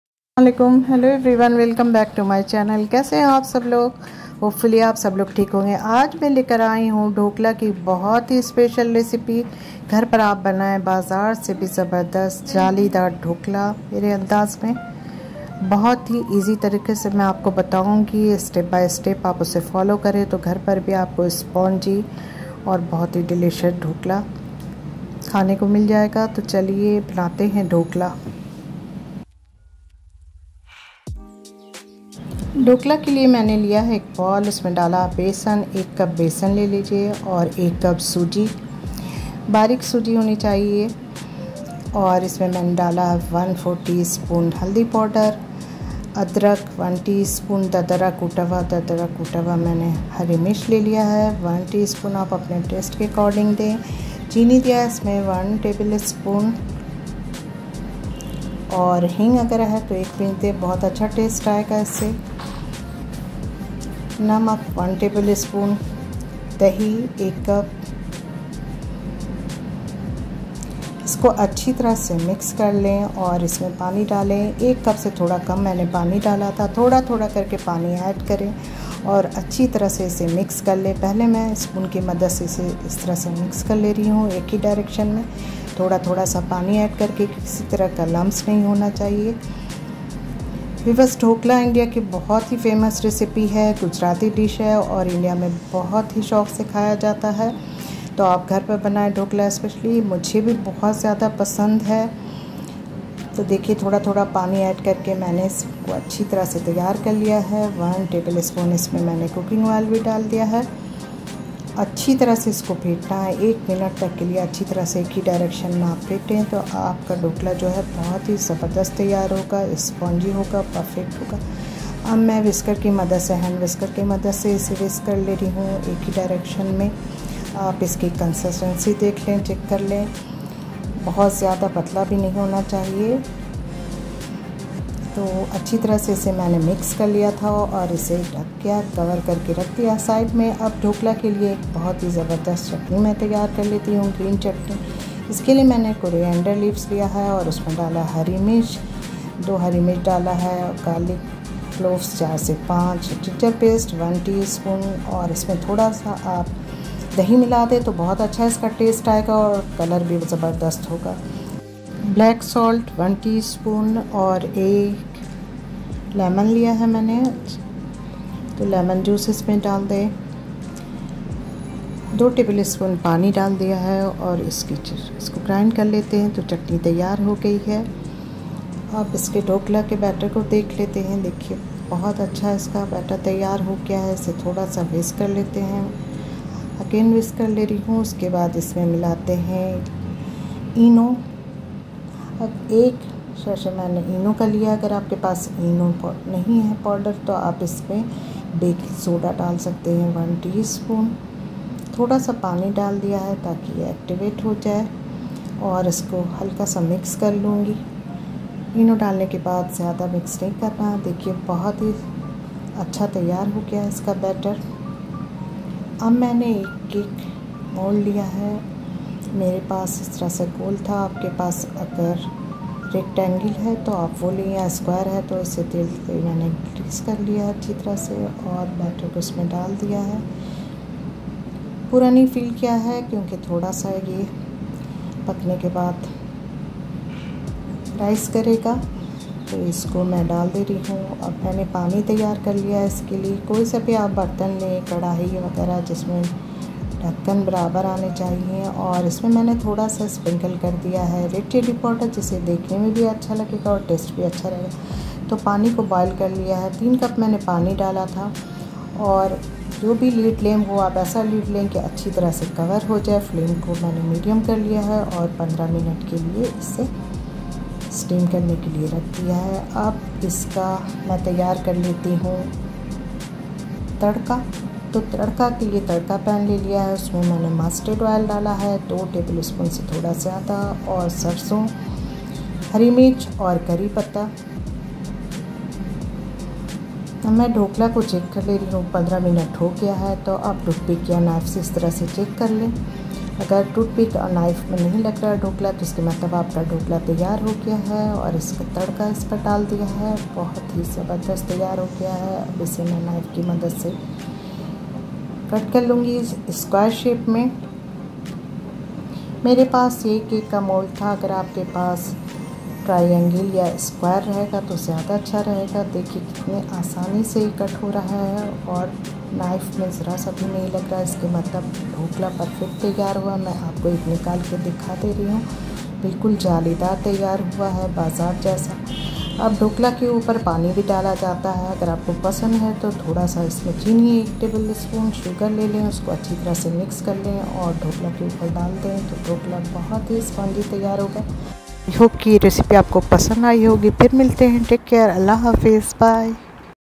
How to make Soft and Spongy Dhokla|A little bit of zaiqa
Ingredients:-

Chickpeas Flour- 1 Cup
Semolina-1Cup
Curd -1 Cup
Termeric powder -1/4 tsp
Ginger paste -1tsp
Green chillies paste -1 tso
Salt-1tbsp
ENO-1 pak